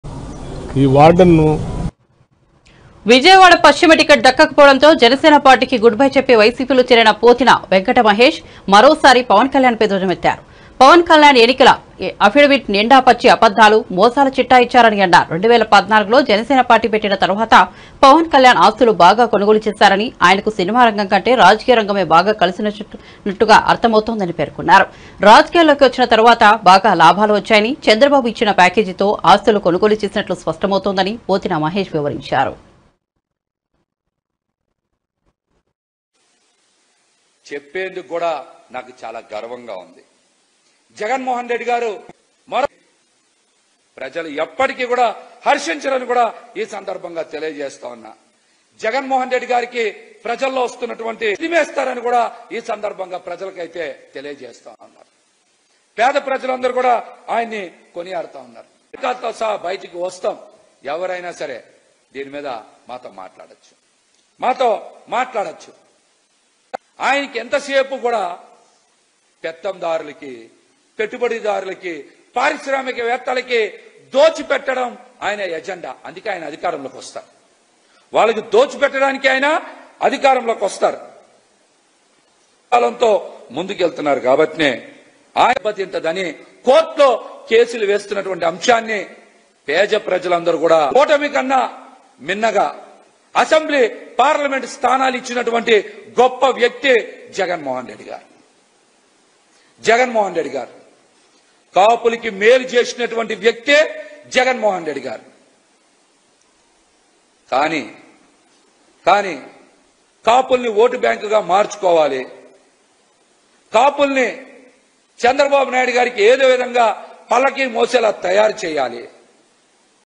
Former Janasena Member Accuses Pawan Kalyan of Lies and Fraud in Election Affidavit
Pothina Venkata Mahesh, who switched allegiance from Janasena to YCP, lashes out at Pawan Kalyan, alleging falsehoods in his election affidavit. Mahesh claims that Kalyan amassed properties after forming Janasena in 2014, suggesting a shift from the film industry to politics for personal gain. Accusing Kalyan of benefiting from Chandrababu's support, Mahesh raises questions about the source of Kalyan's wealth. Delve into the growing rift within Andhra Pradesh politics as allegations fly between former party members.